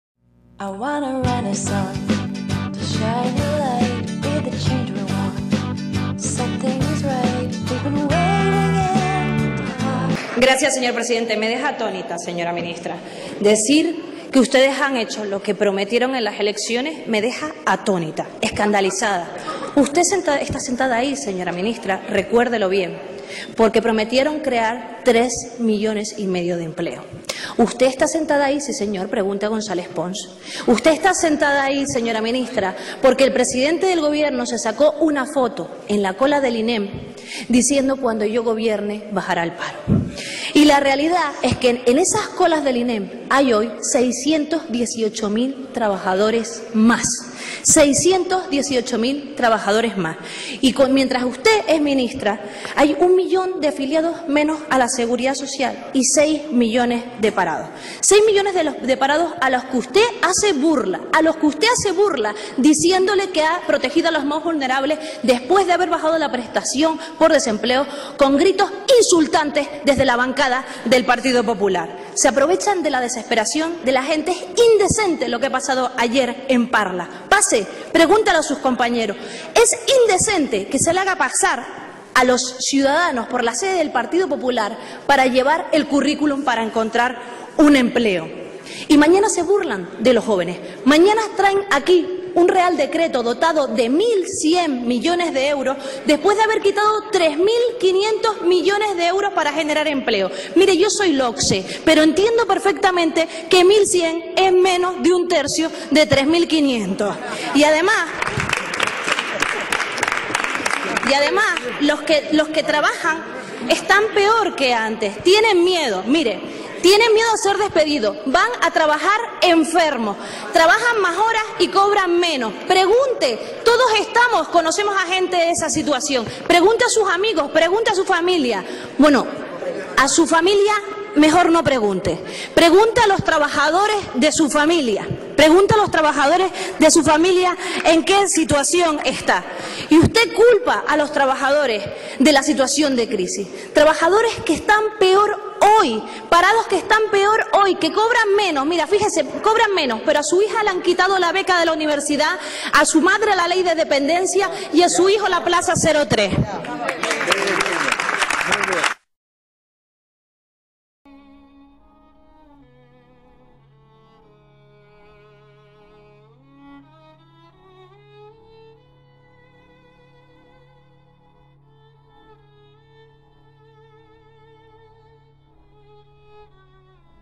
Patricia Hernández "Me escandaliza, decir que han hecho lo que prometieron"
"Me escandaliza, decir que han hecho lo que prometieron... Pregúntele a Pons, que prometió crear 3,5 millones de empleos"
La diputada del PSOE que puso contra las cuerdas a Báñez con aquello de que 'España no se merece una ministra de Trabajo que prefiere los canapés a las cumbres de empleo', no se achanta
La diputada del PSOE Patricia Hernández ha vuelto a protagonizar un enfrentamiento con la ministra de Trabajo, Fátima Báñez, por su hipocresía con las políticas de empleo durante la sesión de control al Gobierno.

Política
España Comunidades Autónomas Internacional